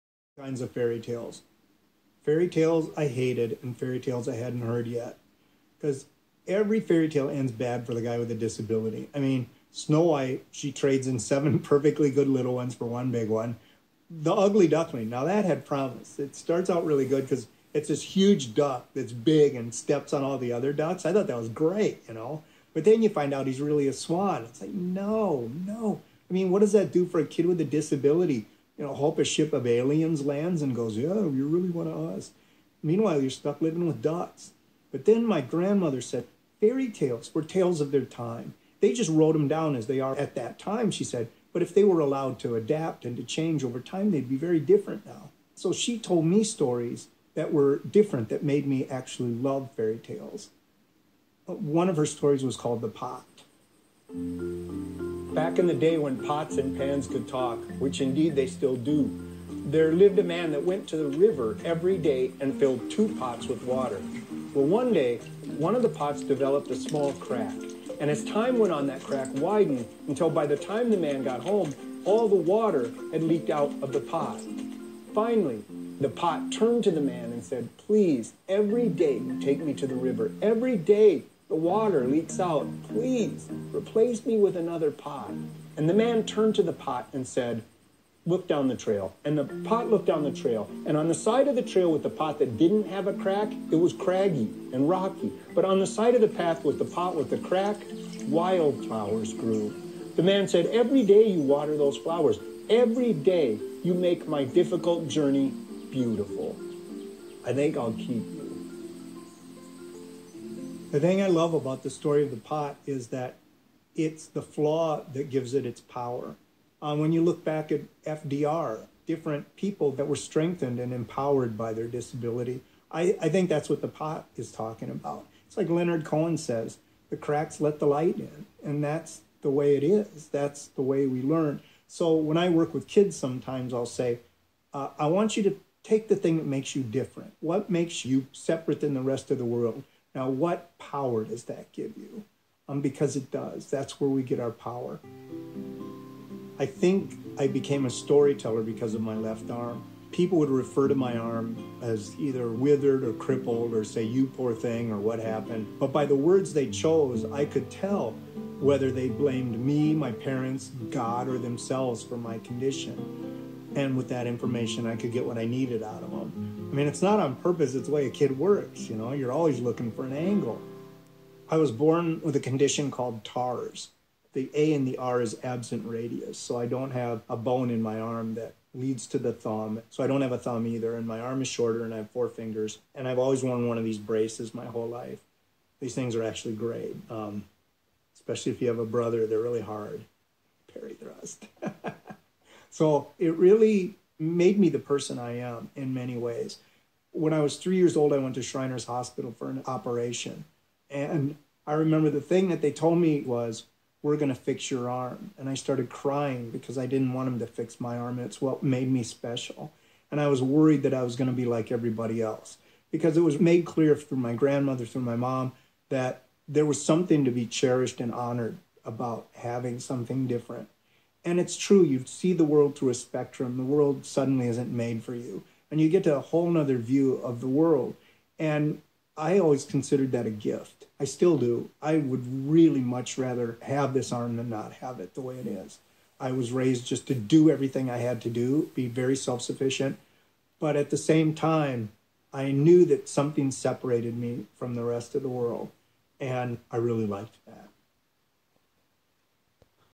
Kevin Kling - The Story of The Pots
Disabilities are,according to Dante, the underworld. It is the place of shadow and reflection where you round off the rough edges of torment and desire. You go to the world of DIS. And it's the prefix for disability, which doesn't mean un-ability, it means able through the world of shadow and reflection.